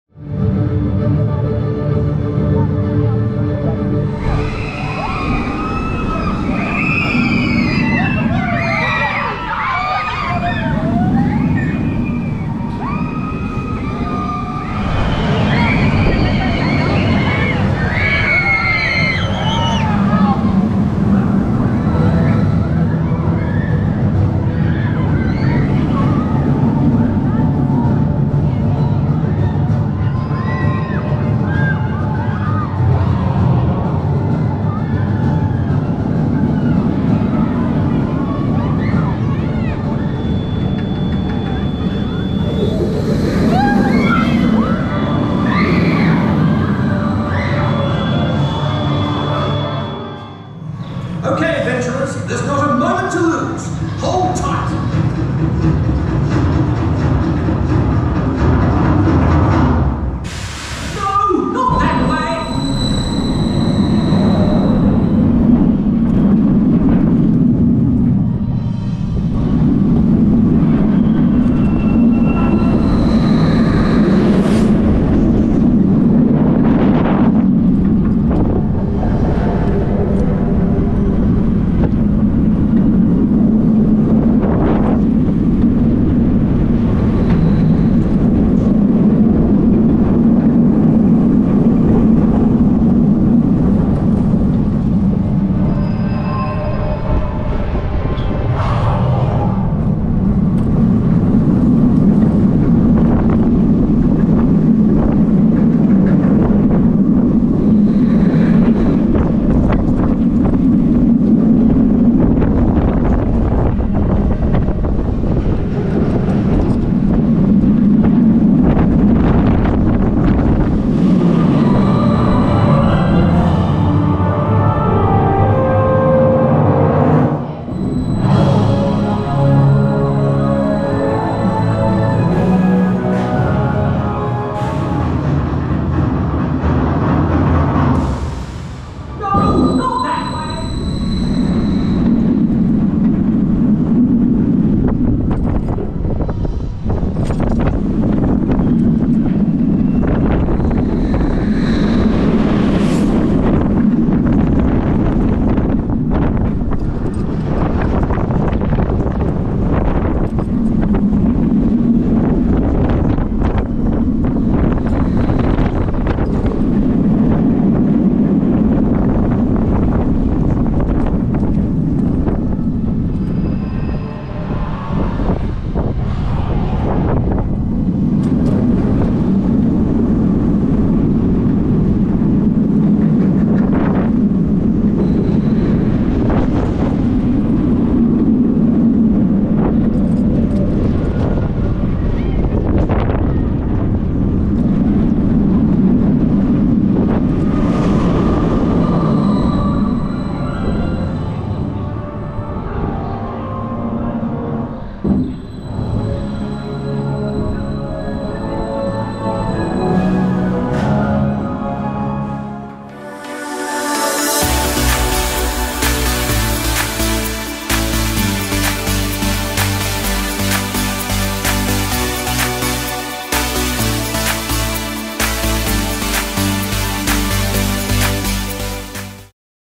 Mandrill Mayhem [4K] Front & Rear Seat POVs - World of Jumanji - Chessington World of Adventures
Mandrill Mayhem is the first winged shuttle coaster designed by Bolliger & Mabillard, and the flagship attraction in the new for 2023 World of Jumanji at Chessington World of Adventures Resort. Riders experience four launches on a thrilling journey around the highly-themed tropical land, including a close encounter with the mighty Jaguar Shrine.

Filmed with permission from Chessington World of Adventures Resort in a controlled environment.

AttractionSource.com | TowersTimes.co.uk